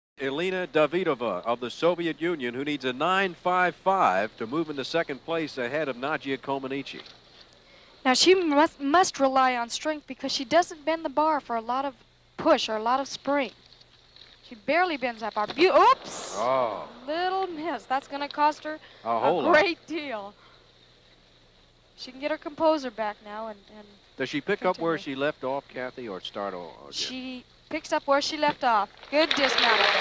Before They Were Stars ✨- Elena Davydova (Елена Давыдова) Bars
The Soviet Union sent 14 year old Elena Davydova to the 1976 American Cup to gain valuable experience. She had some struggles and finished in third place--over 3 points behind the soon-to-be Olympic AA Champion, Nadia Comaneci.
Four years later, the tables would turn and it would be an 18 year old Elena who would triumph over Nadia to win the 1980 Olympic gold medal 🥇
I love the back tuck skill she does over the low bar in this routine 😍